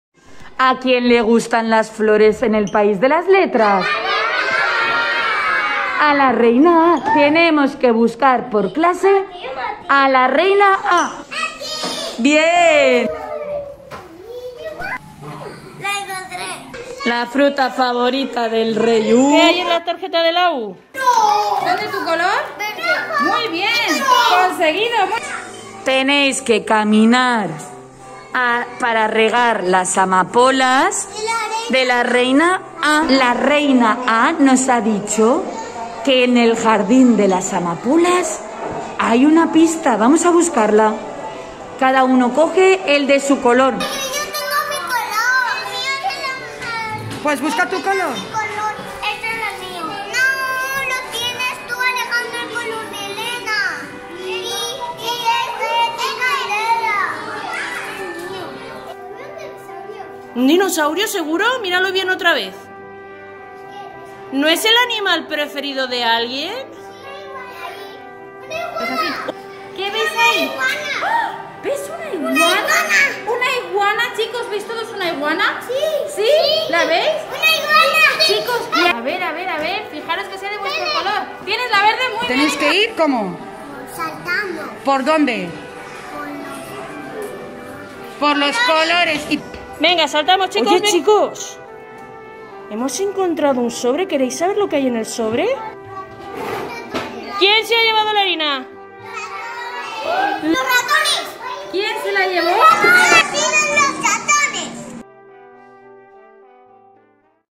escaperoom misterio aprenderjugando educación infantil
2infantil ha sido invitado a solucionar un misterio. Ha desaparecido la bolsa de harina de la cocina de la reina A. ¿Quién habrá robado la harina? ¿Lo podrán descubrir?